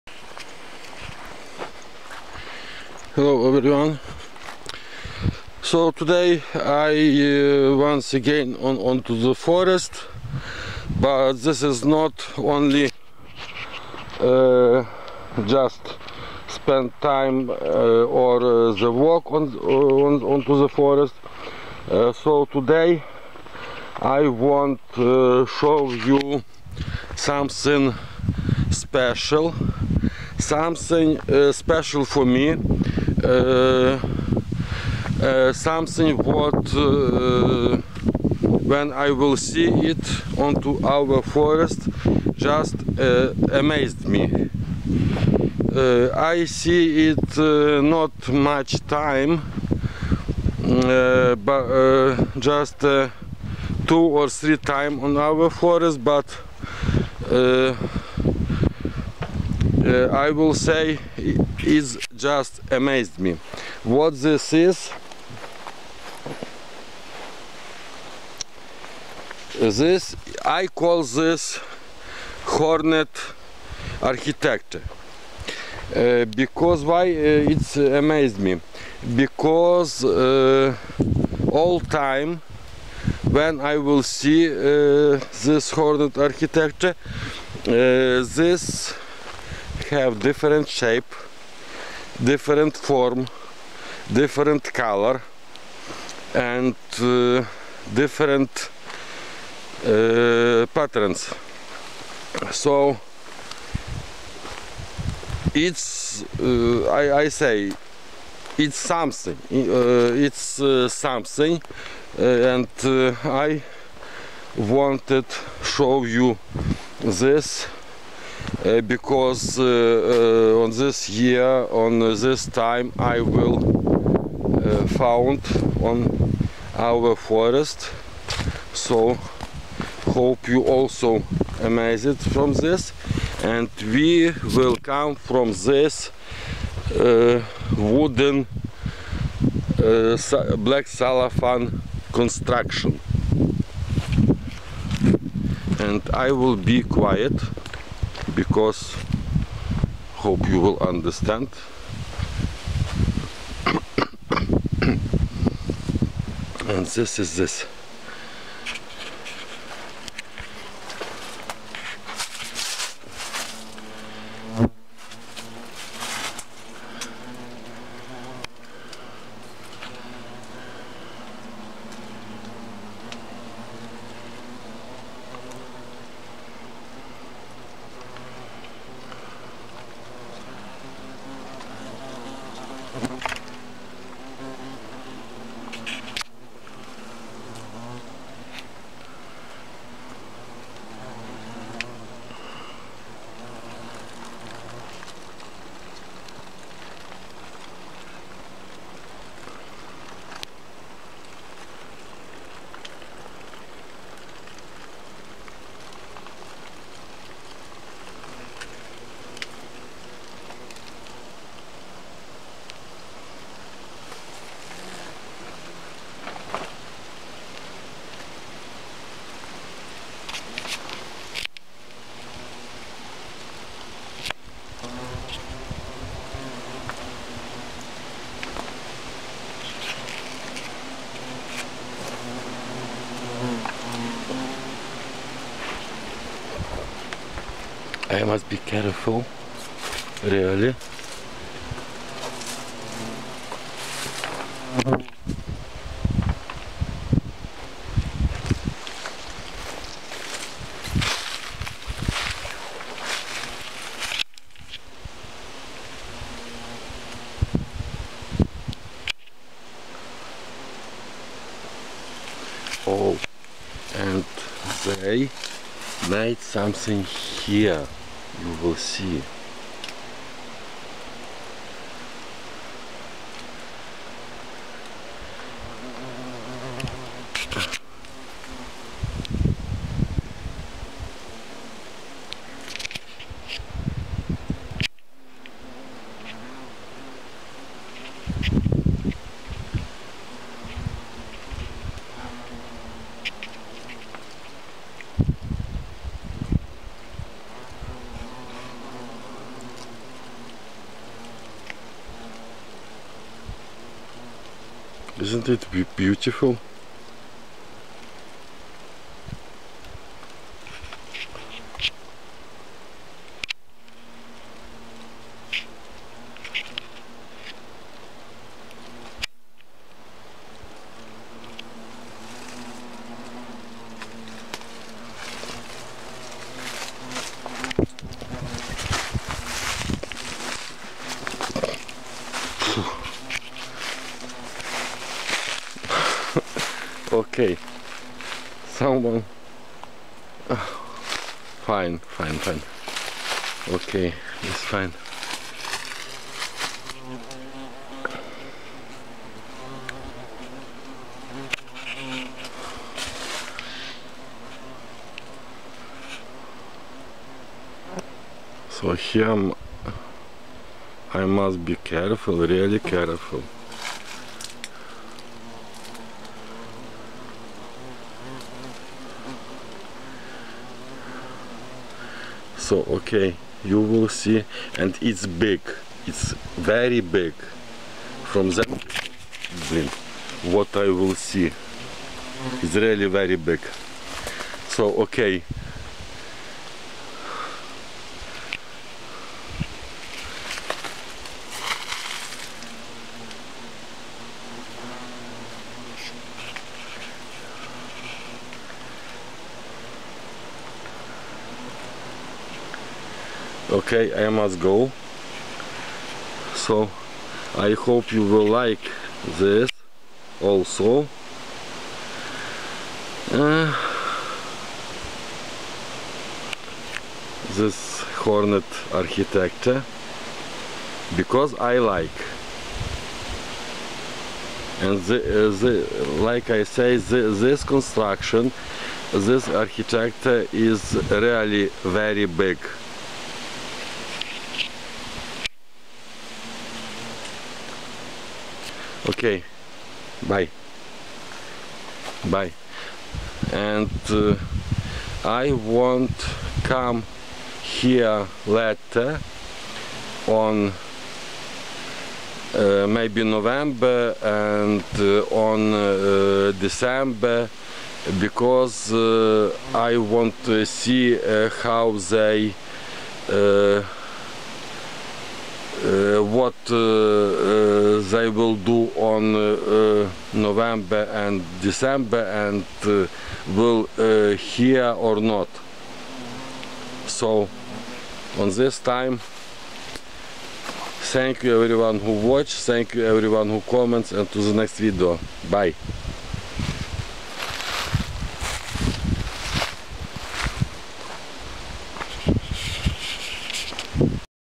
Hornet architecture
Several hornets attacked me, but everything is in order. No harm done. And me too.
In fact, hornets are dangerous creatures. Their bite is not fatal, but if it bites more, it's bad. They are especially dangerous for people who have allergies and it is not particularly important to be allergic to what.